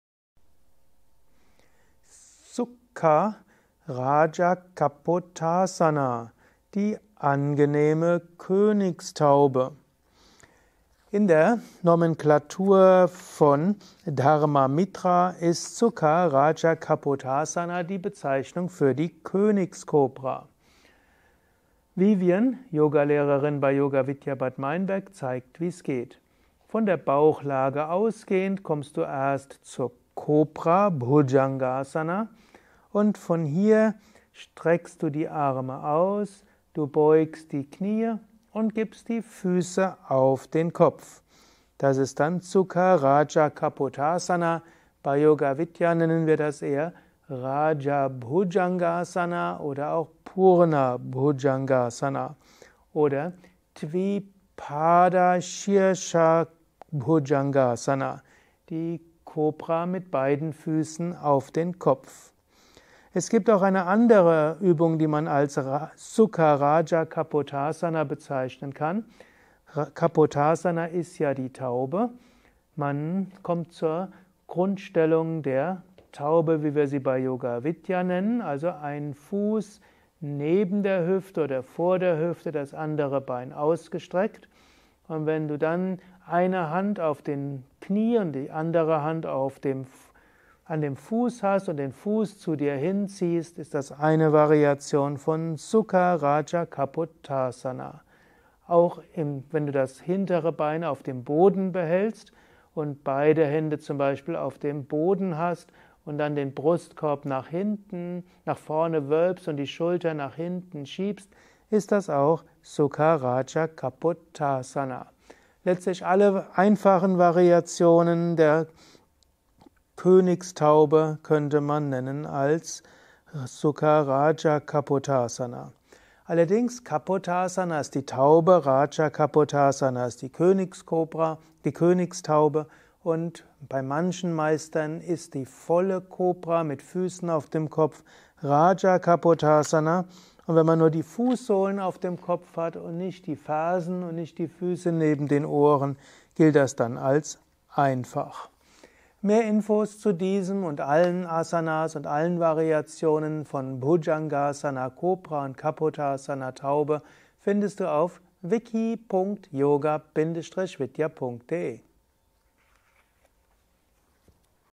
Sukha Rajakapotasana - Asana Lexikon 2890
Sukha Rajakapotasana bzw. सुखराजकपोतासन, sukha-rāja-kapotāsana: Erläuterung, Vorführung und Übungsanleitung. Sukha Rajakapotasana gehört im Yoga zu den Rückbeugen, Liegende Asanas, Asanas in Bauchlage, Asanas im Sitzen.

Sukharajakapotasana ist eine Variation von Kapotasana